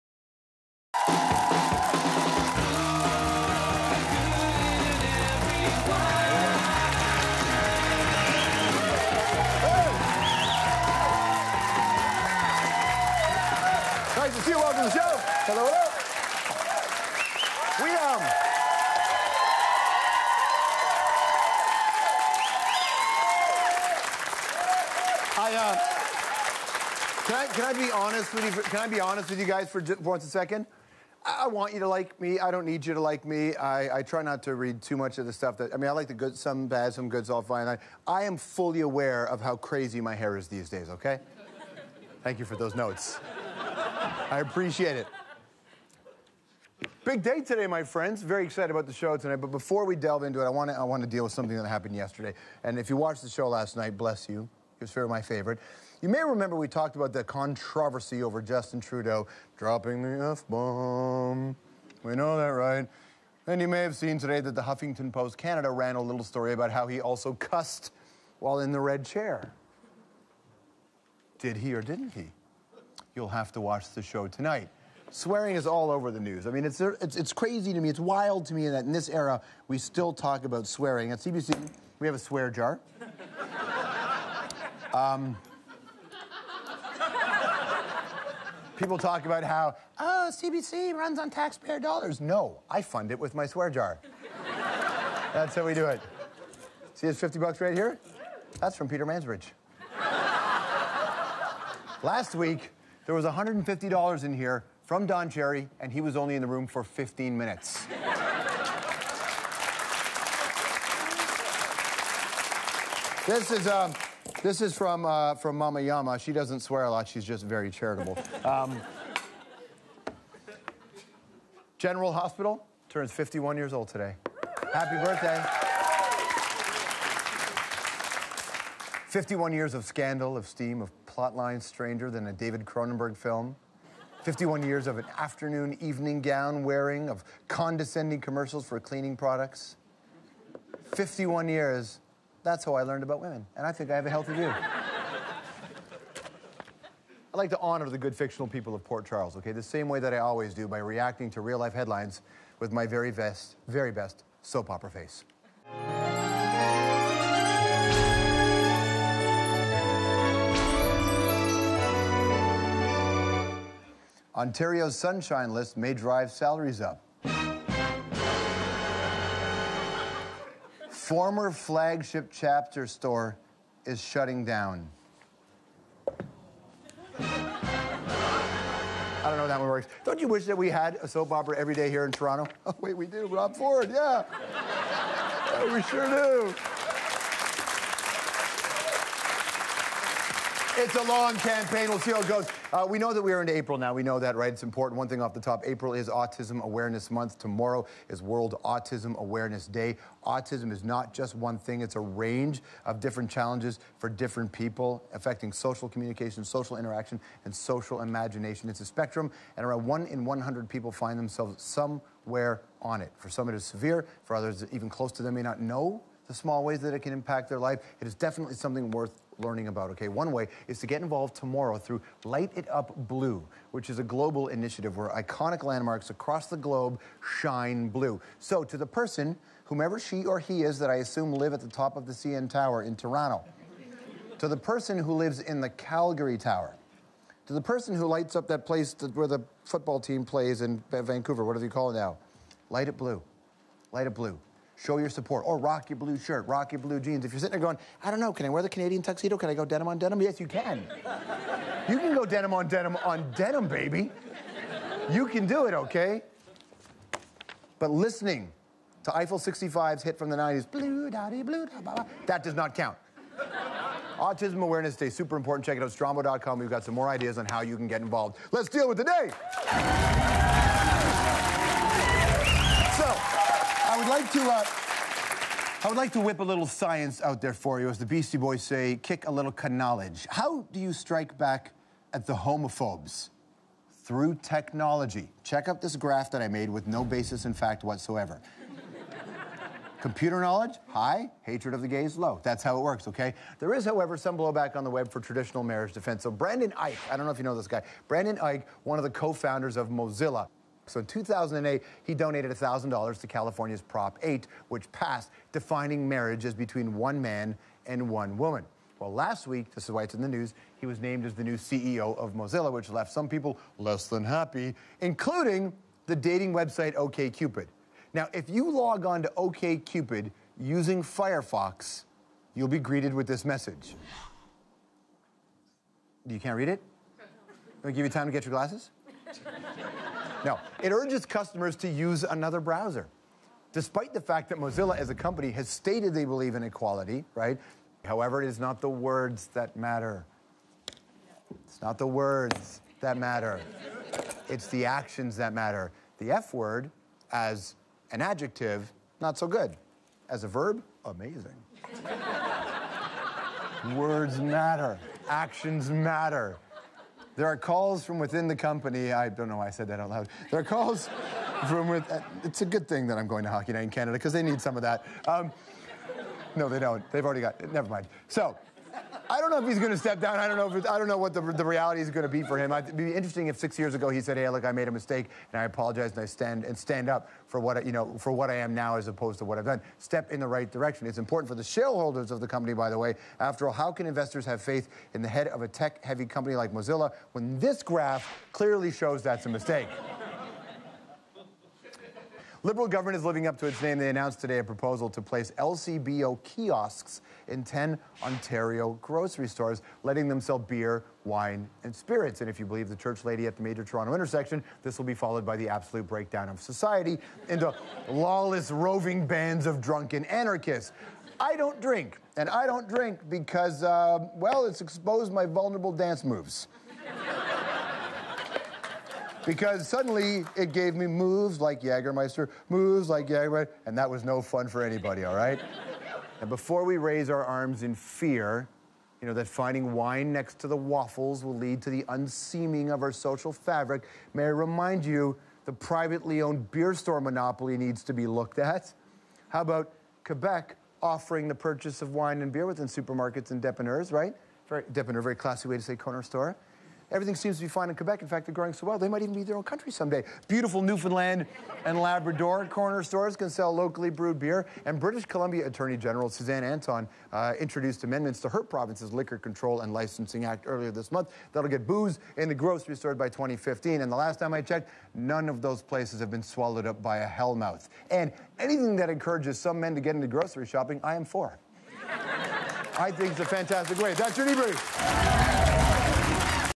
Debrief For April 1, 2014: Booze In Ontario Grocery Stores?
For this edition of The Debrief George talked about General Hospital's 51st anniversary, the CBC swear jar, World Autism Day and OKCupid vs. Mozilla.

credits

DRAMATIC SOUND - Youtube user Dark Bowser

BRENDAN EICH - Mozilla Press Center

SYDNEY BLUE - Richard Milnes